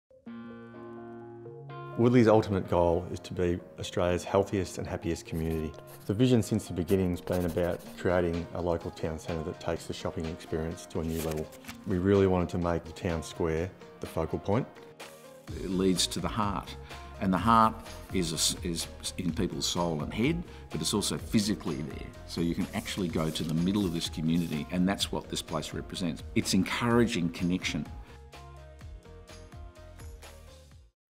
The Vison of Woodlea Town
Meet the minds who brought this $45M precinct to life! Woodlea Town is set to come a destination for the West, not just the Aintree community. Find out the inspiration from each of the designers & how they envisioned the space to function.